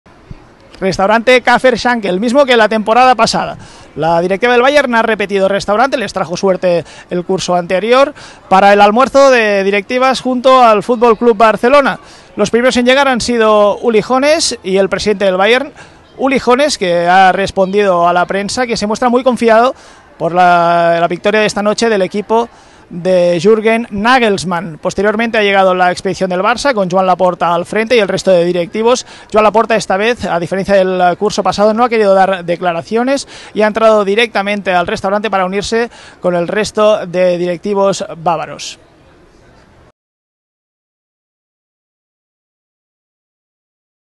La última vez les dio suerte: La directiva del Bayern repite restaurante para reunirse con el Barça
Hoy El Bayern de Munich y el Barça se enfrentan en la Champions League. Las dos directivas se han reunido en un restaurante 'fetiche' para los alemanes.